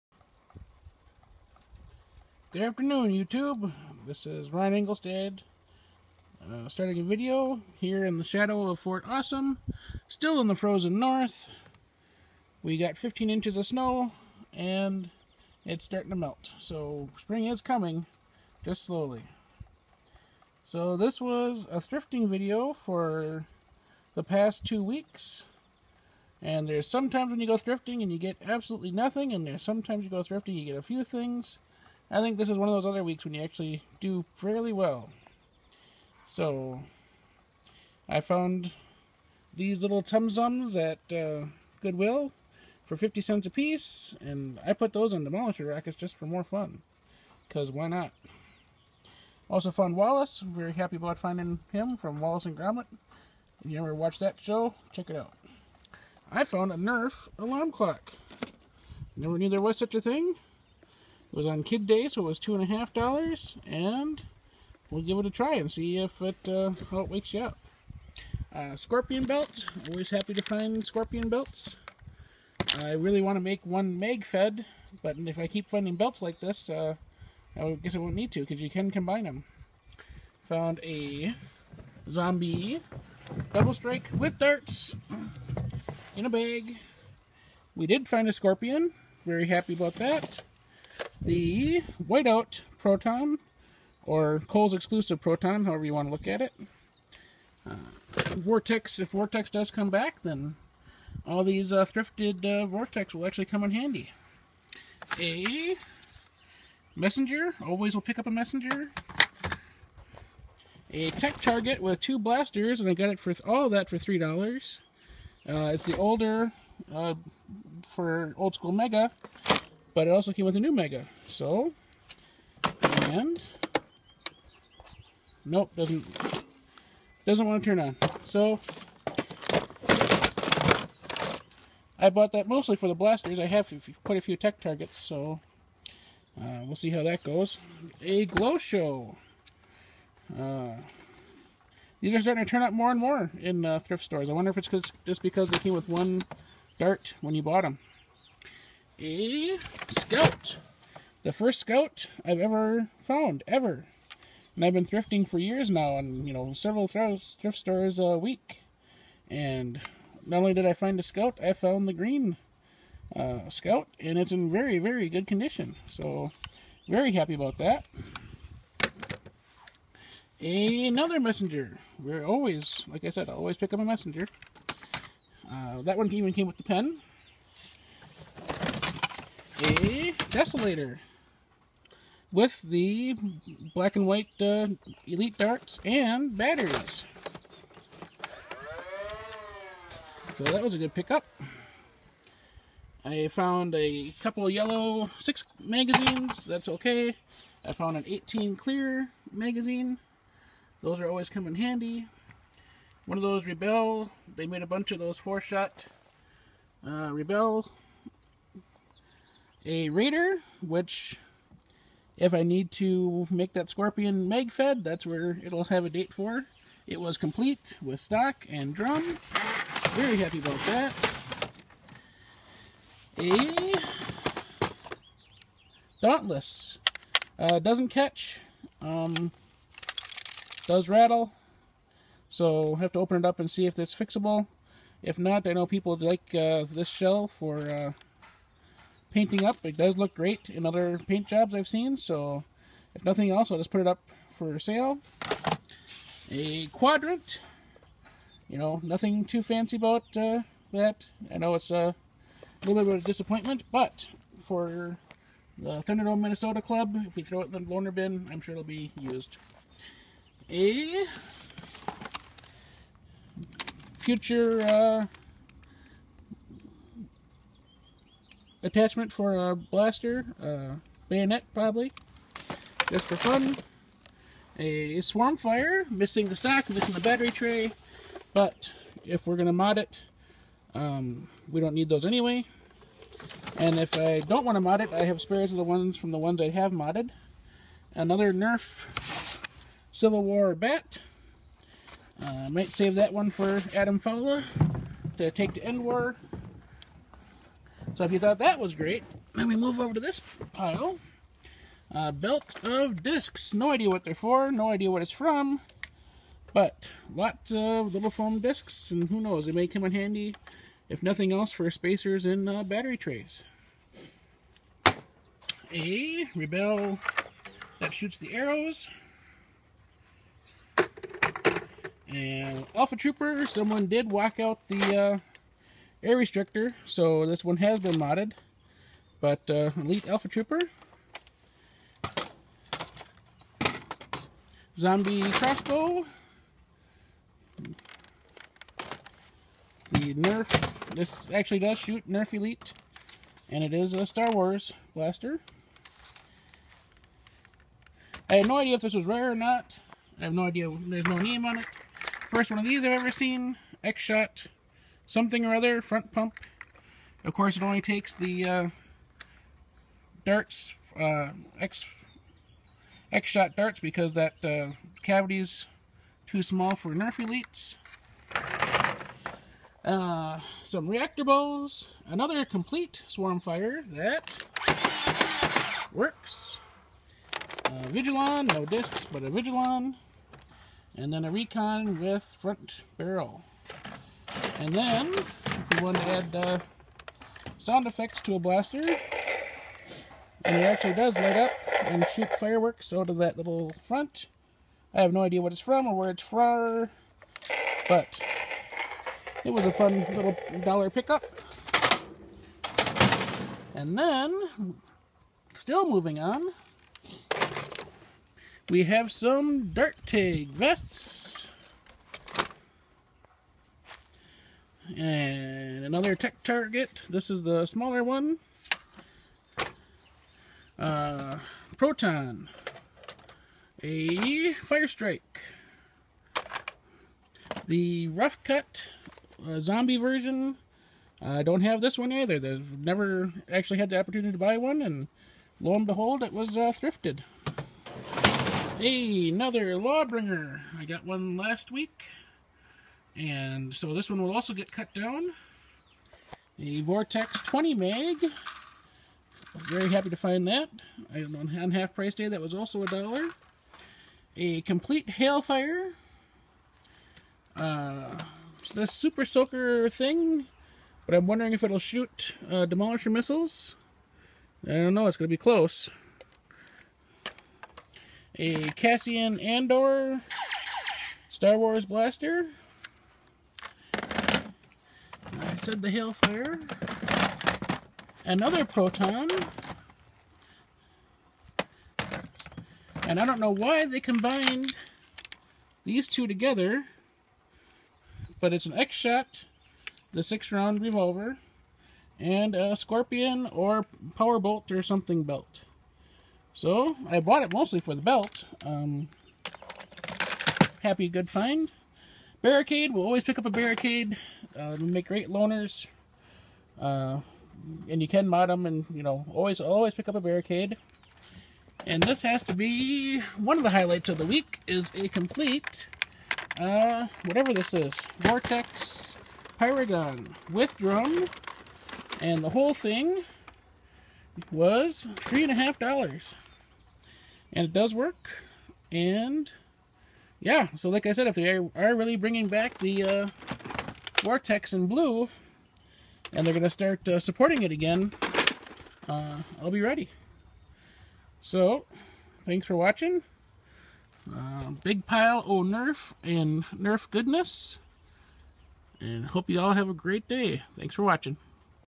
Huge Nerf thrifting haul from early weeks of April 2018
Nerf blasters found thrifting the first few weeks of April including blasters I never found before!